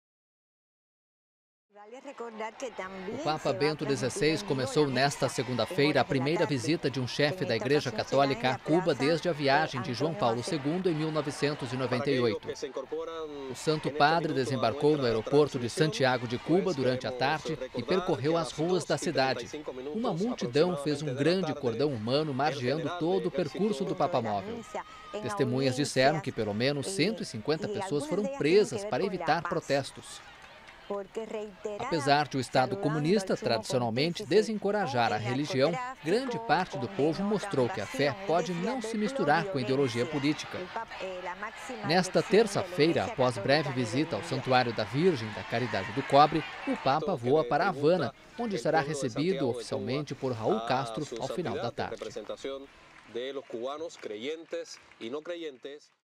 Cubanos recebem papa com alegria
A ilha é comunista. O Estado sempre ignorou a religião. Mas o povo nas ruas vibrou com a chegada do papa Bento XVI nesta segunda-feira a Cuba. Esta é a primeira visita do chefe da Igreja Católica ao país desde João Paulo II em 1998.